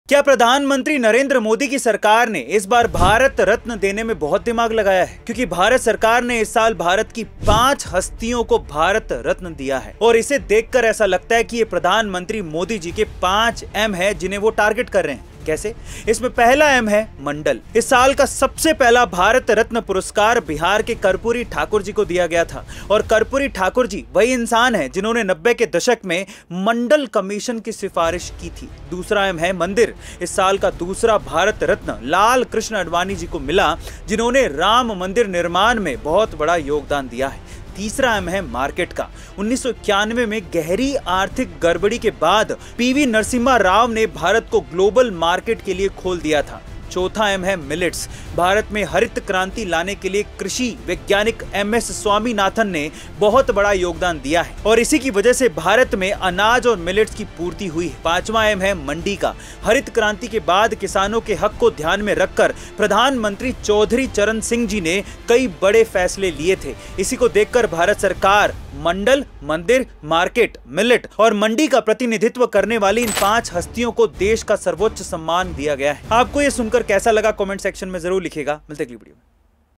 मोदी जी ने भारत रत्न देने में कैसे लगाया दिमाग़🧠! bharatratna narendramodi by Dr.
Has Modi ji put a lot of thought into giving Bharat Ratna this time? The Government of India has given Bharat Ratna to five Indian celebrities this year! And looking at this, it seems that Prime Minister Modi ji has targeted the five M's! How? The first M in this is Mandal! The first Bharat Ratna award this year was given to Karpoori Thakur ji of Bihar, and Karpoori Thakur is the same person who had recommended the Mandal Commission in the 90s! Second M is temple! This year's second Bharat Ratna has been given to Lal Krishna Advani ji, who has made a huge contribution in the construction of Ram Temple. The third M is for Market! After deep economic crisis in 1991, PV Narasimha Rao opened India to the global market. Fourth M is Millets! Agricultural scientist MS Swaminathan has made a huge contribution in bringing green revolution in India. Due to this, there was supply of grains and millets in India. The fifth M is of market! After the Green Revolution, Prime Minister Chaudhary Charan Singh had taken many big decisions keeping in mind the rights of the farmers. Seeing this, the Government of India has given the country's highest honor to these five personalities representing Mandal, Temple, Market, Millet and Mandi.